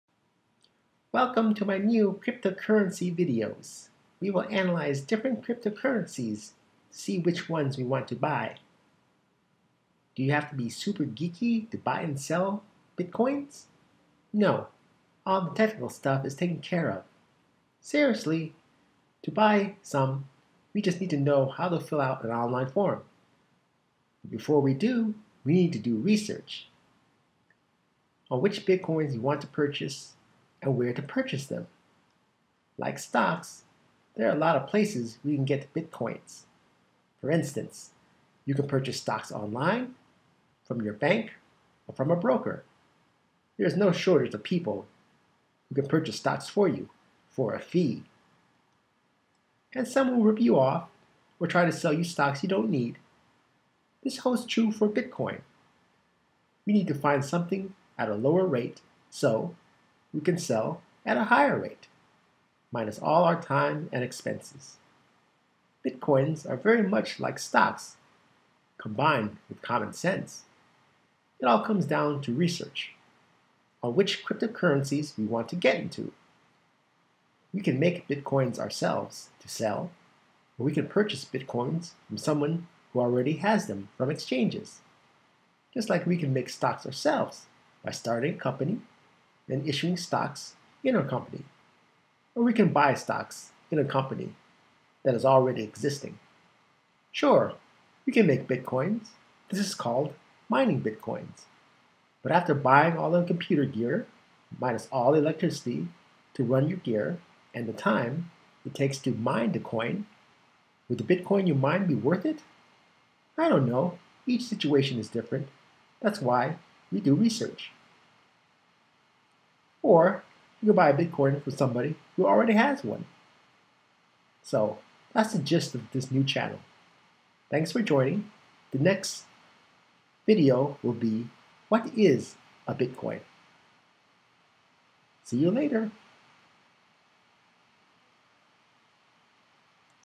Bitcoin CryptoCurrency beginners tutorial
Introduction to beginning cryptocurrency tutorial. Created with Adobe Illustrator, Character Animator, Adobe After Effects and Premier.